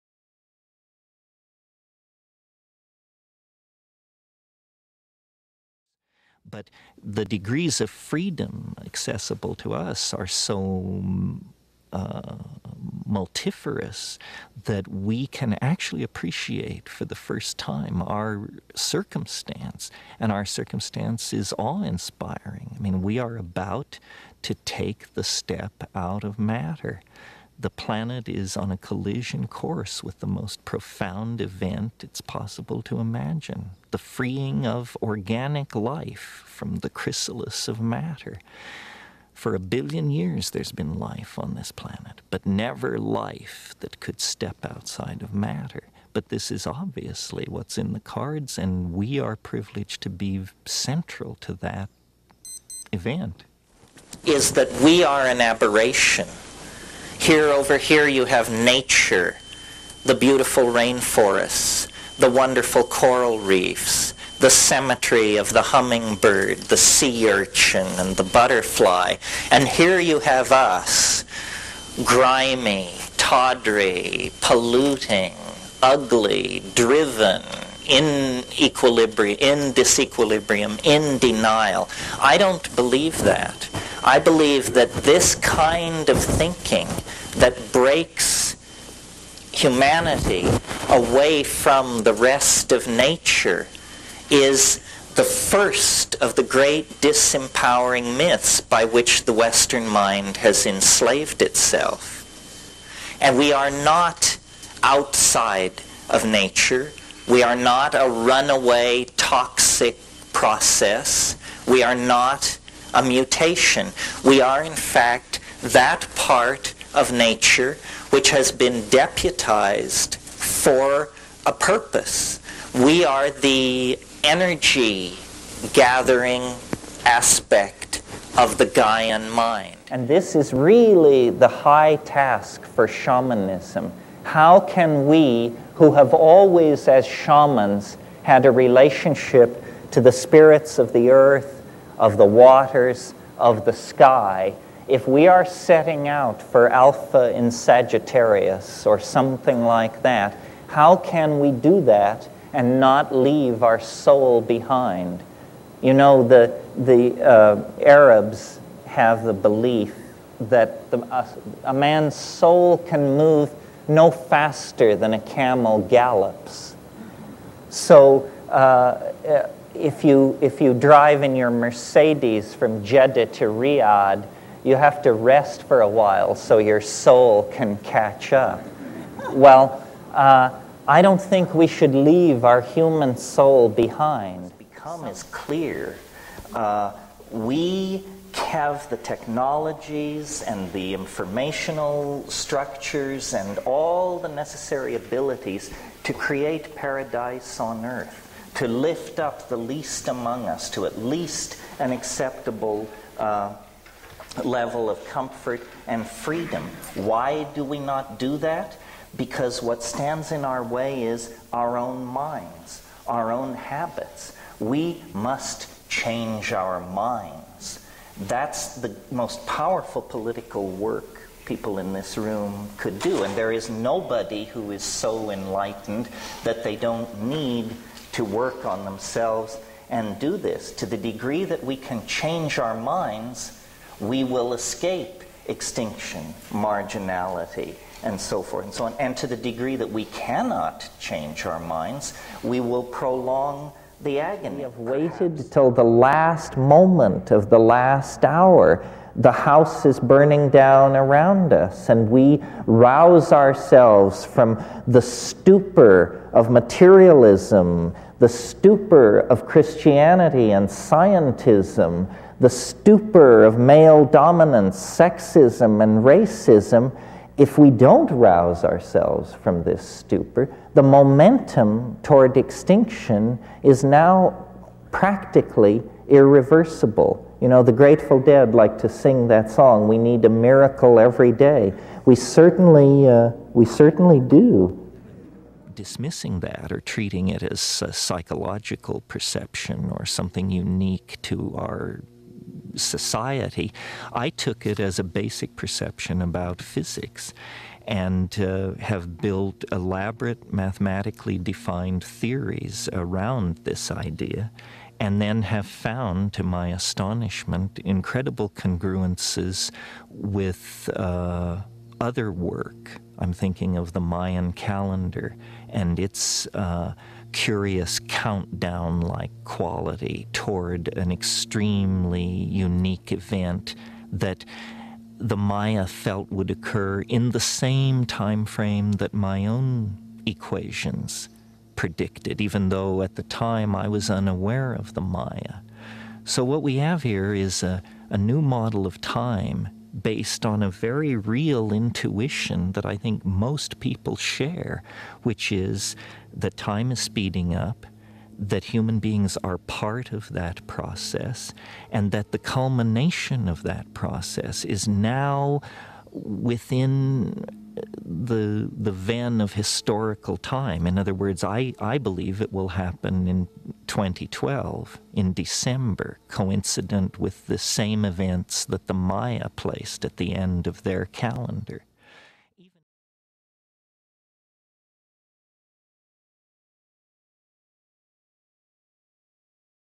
Terence McKenna Mix ~ Messages for The Future
Terence Mckenna shares wisdom for us today.  These clips are all from the 90's.